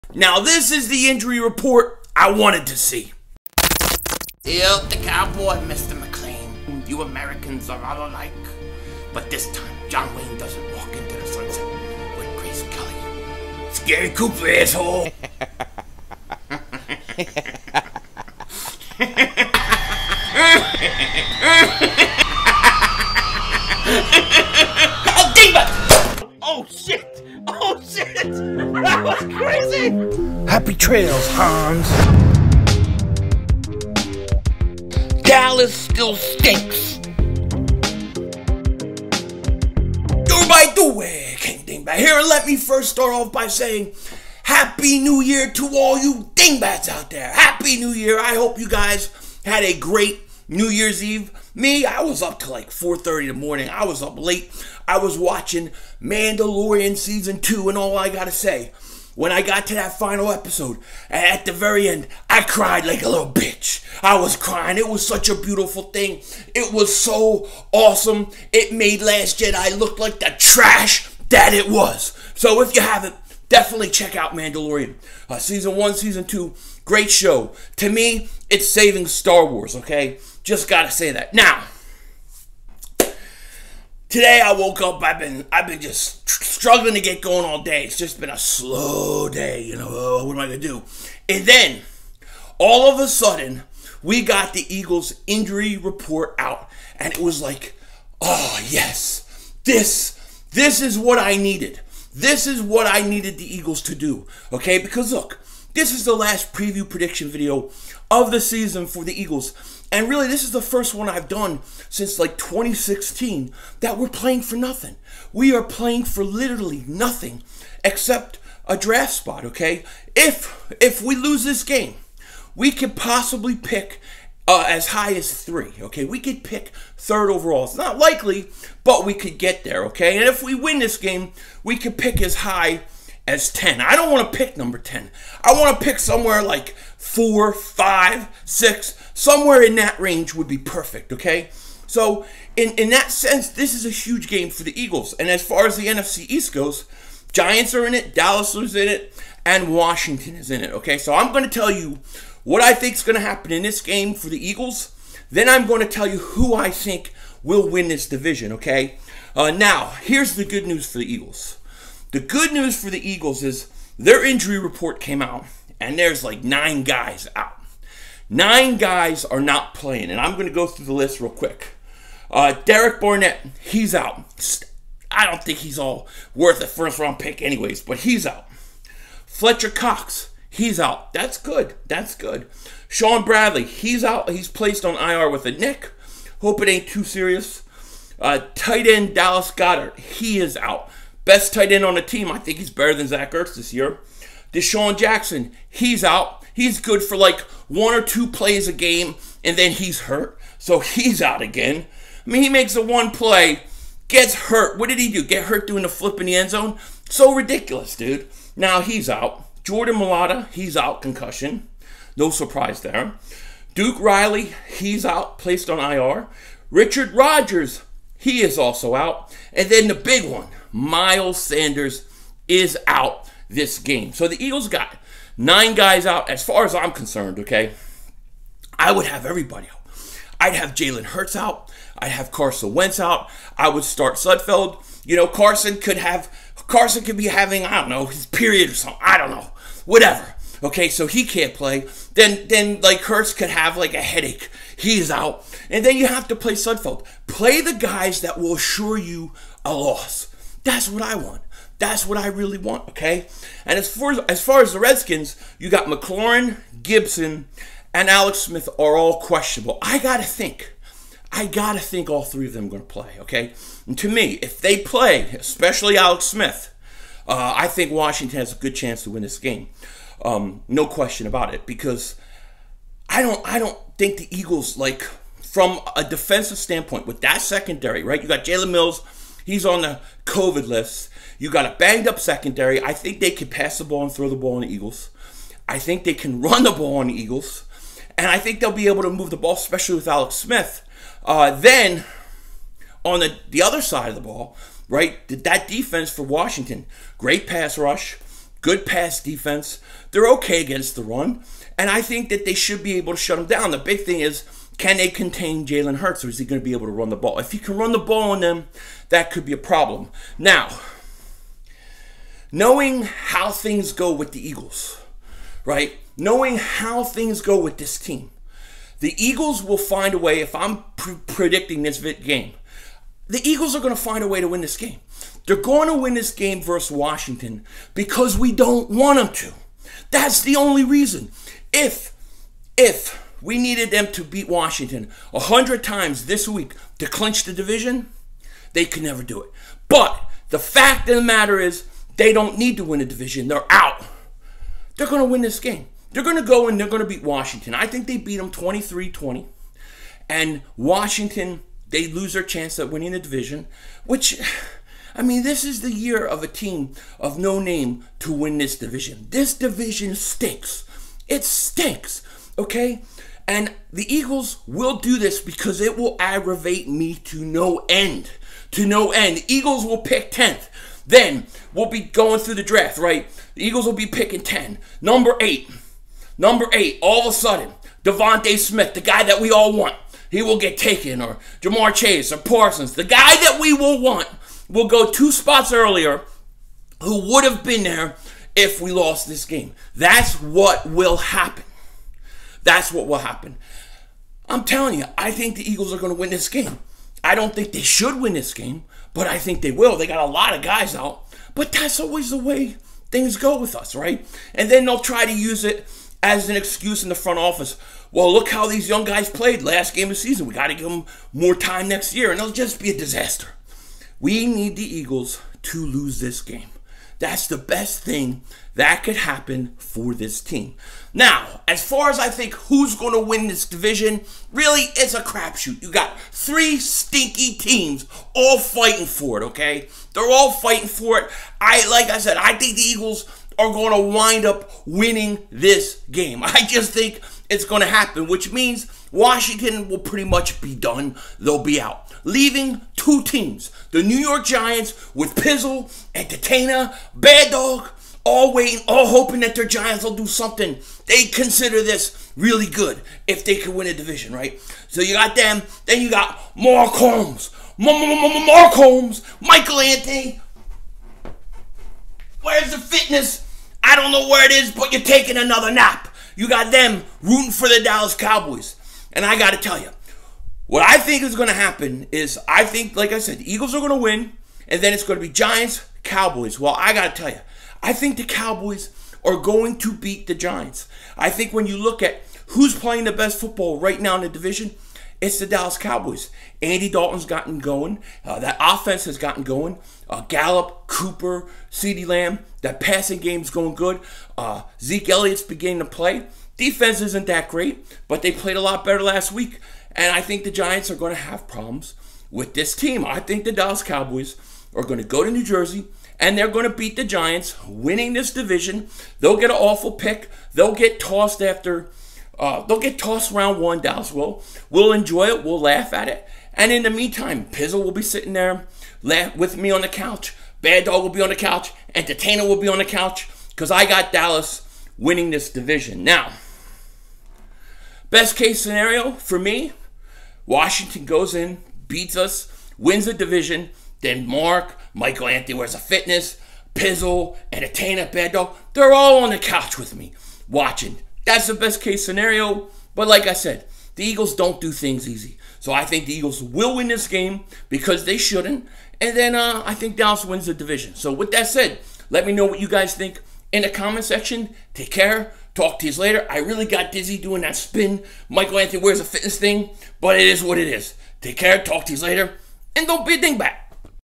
Eagles Need That Top 5 Pick | Miles Sanders And 8 Others Out | NFC East Division Prediction!!!
Who will win the NFC East ? not the Eagles but we could potentially get a top 5 pick!!!